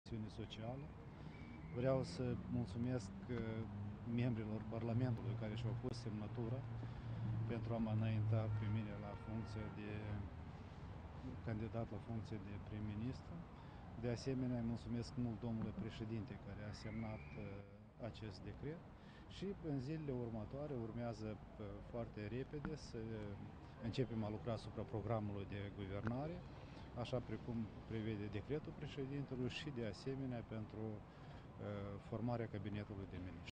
Desemnarea lui Pavel Filip în calitate de candidat pentru funcția de prim-minist...
Pavel Filip, al 3-lea candidat la funcția de prim-ministru.
Titlul evenimentului: Desemnarea lui Pavel Filip &#238;n calitate de candidat pentru funcția de prim-ministru